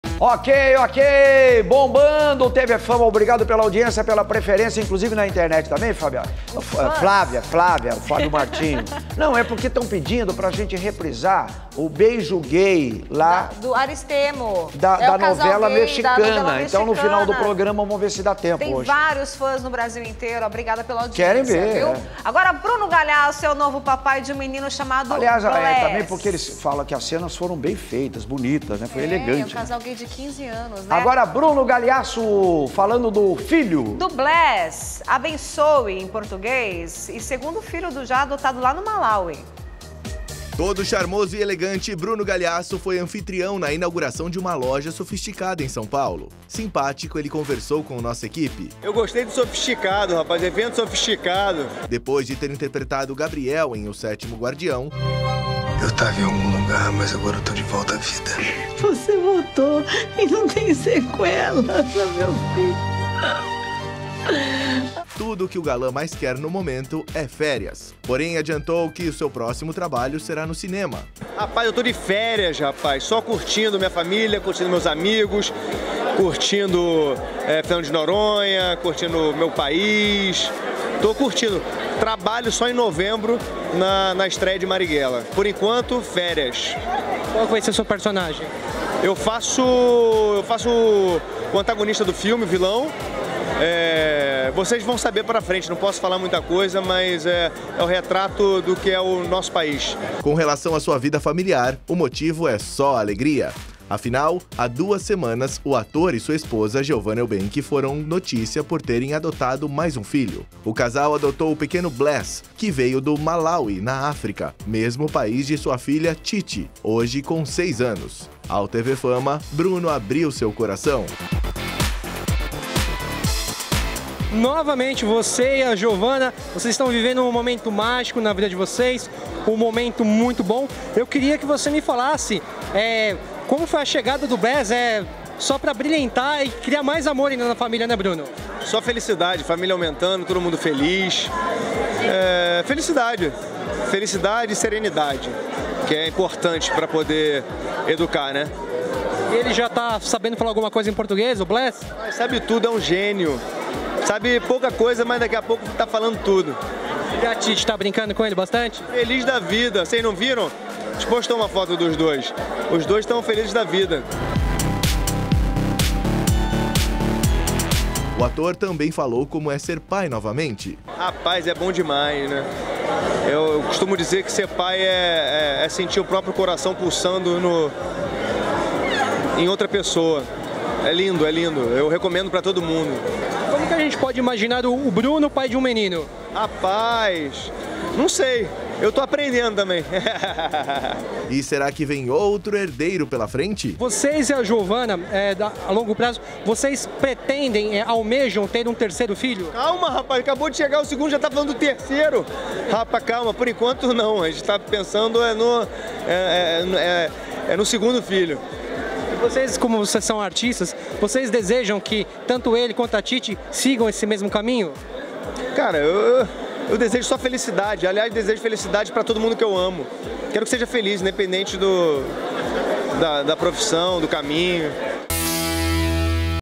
"Sentir o próprio coração pulsando em outra pessoa", diz Bruno Gagliasso sobre ser pai
Bruno Gagliasso conta sobre como está sendo a nova rotina com o filho Bless, de apenas quatro anos."Só felicidade, família aumentando e todo mundo feliz, felicidade e serenidade que é importante para educar", disse o ator, que também é pai de Titi, de seis anos. Inclusive, ele revela que Bless é um gênio e está aprendendo com facilidade o português.  Sobre ser pai, ele fala: "Ser pai é sentir o próprio coração pulsando em outra pessoa". De férias, só curtindo a vida e a família, Bruno adianta: "trabalho só em novembro".